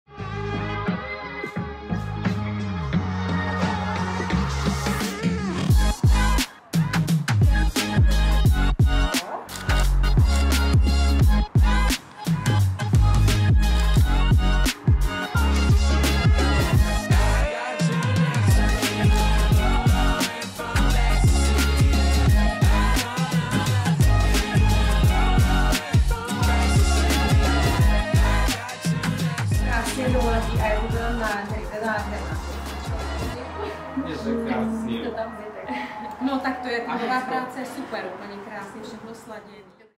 VUCH | Backstage z focení s Luckou Borhyovou
Zajímá Tě, jak vznikaly všechny ty hvězdné vizuály, které jsme vytvořili s naší novou ambasadorkou, Luckou Borhyovou? ✨ Nahlédni pod pokličku jednoho velkého focení. Za tím vším, co vidíš, stojí spousta plánování, kreativních nápadů a času, ale jak vidíš, stojí to za to. S Luckou se to navíc fotí úplně samo, a hlavně s úsměvem. 🤍📸